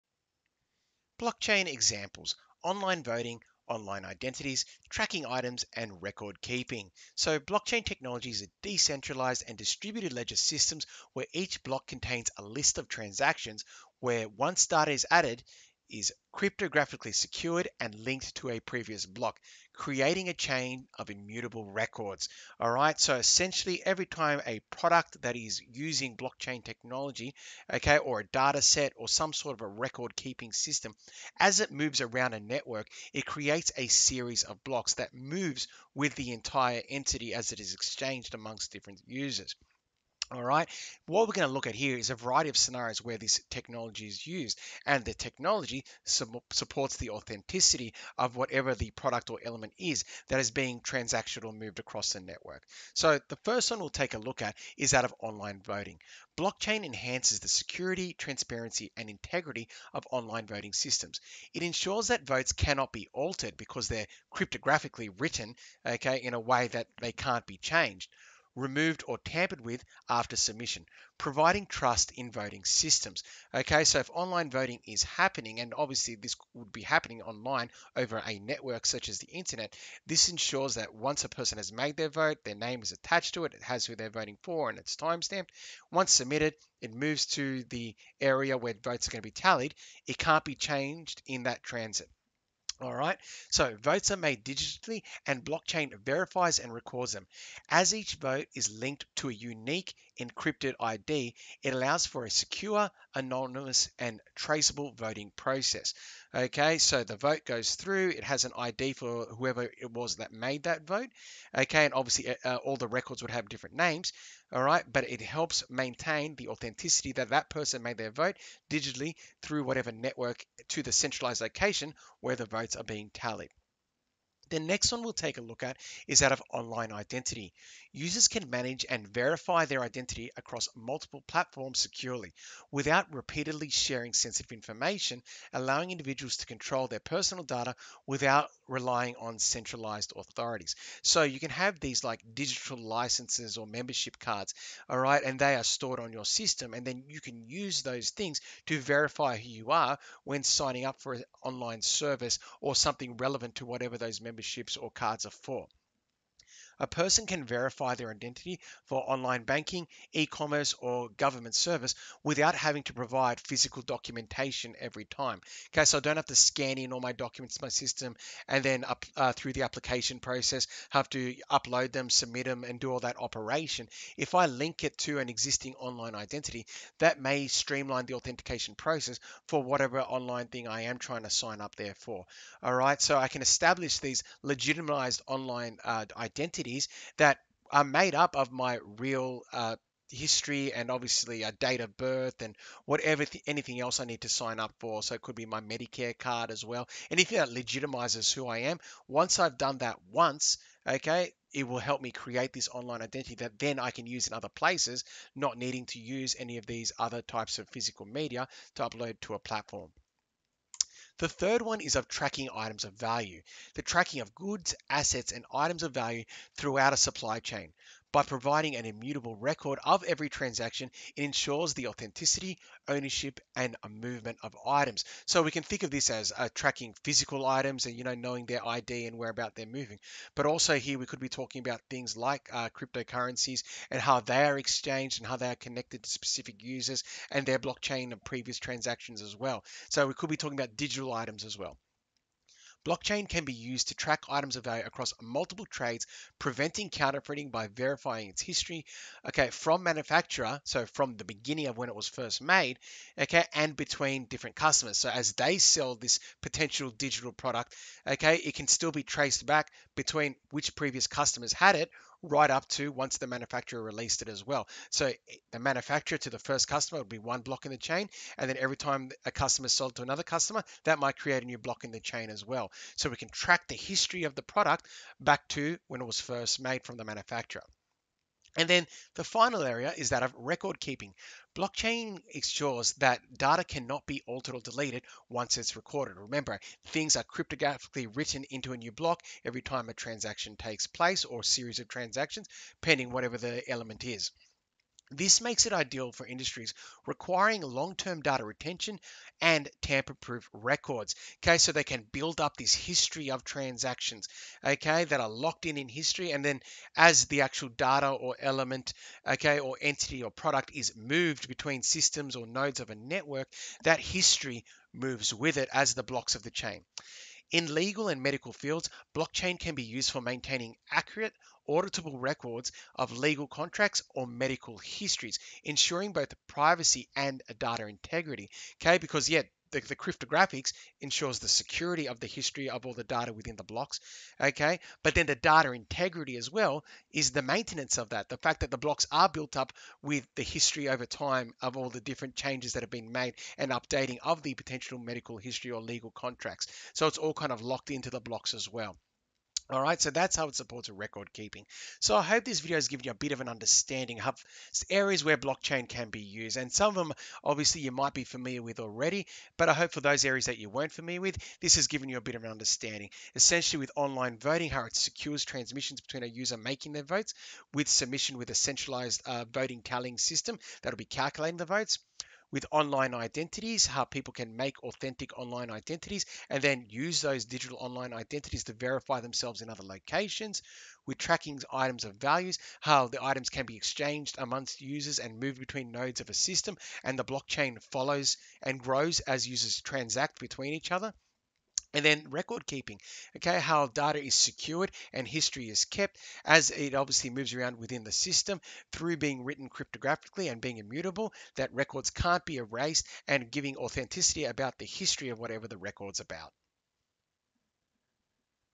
Examples Of Blockchain Technology
Blockchain technology is a decentralised and distributed ledger system where each block contains a list of transactions, where once data is added, are is cryptographically secured and linked to the previous block, creating a chain of records.

Examples of areas where Blockchain technology is used include:
- Online Voting
- Online Identities
- Tracking Items of Value
- Record Keeping